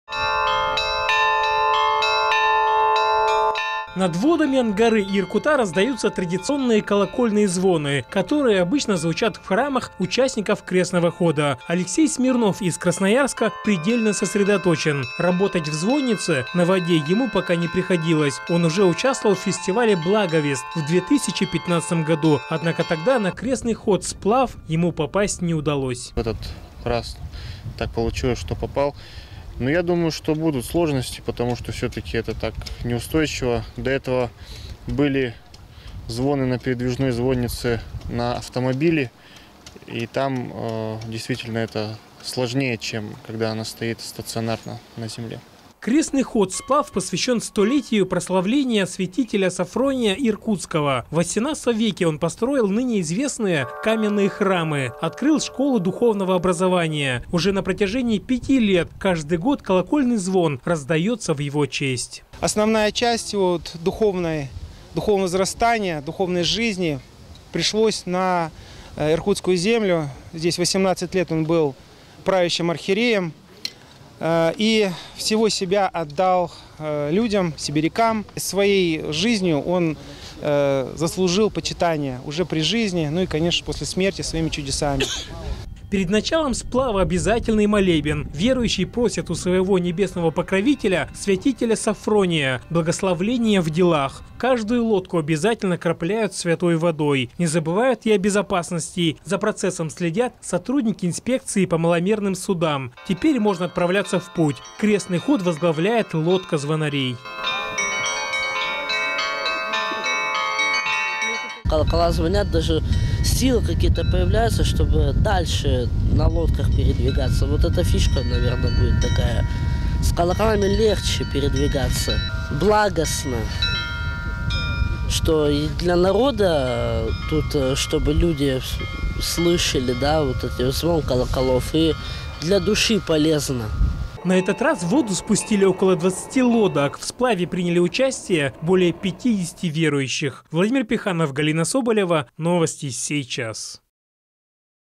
Крестный ход
В крестный ход на лодках. Более 50 верующих сплавились по рекам Анагара и Иркут. Так в Приангарье отметили 100 лет со дня прославления Святителя Софрония Иркутского. Достаточно ли было веры для преодоления дистанции, расскажет моя коллега.